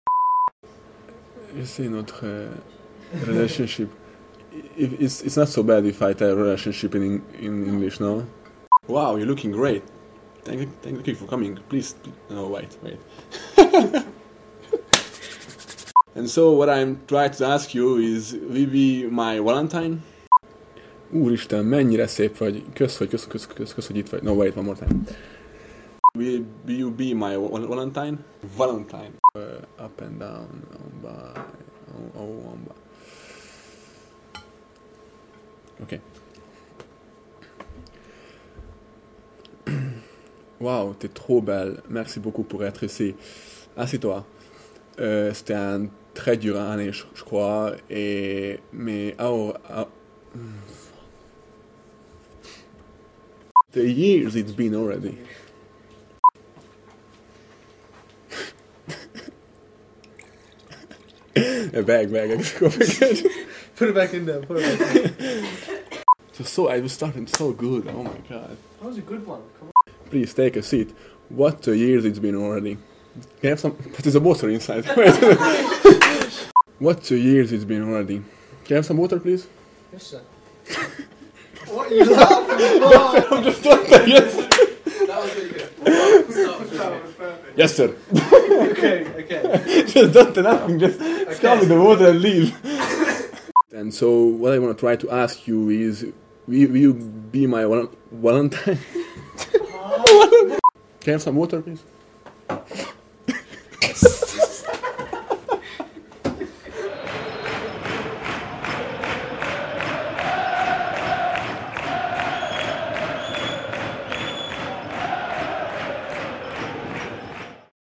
FUNNY - Portsmouth FC's Marko Futacs Valentine's Bloopers
See all of the outtakes from Marko Futac's Valentine's message. Your Club Needs You.